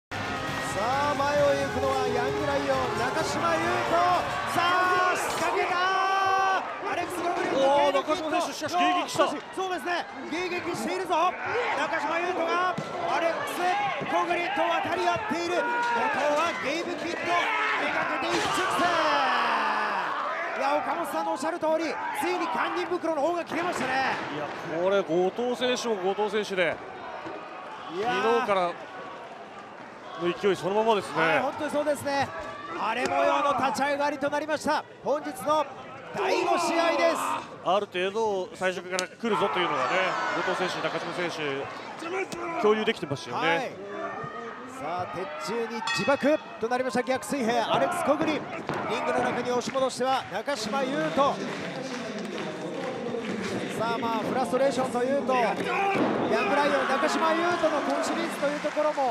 新日本プロレス『NEW JAPAN ROAD ～ Road to STRONG ～永田裕志Produce Blue Justice Ⅻ ～青義回帰～』（6.18） 第5試合
6月18日(日) 千葉・佐倉市民体育館

試合の模様は 新日本プロレスワールド で配信中！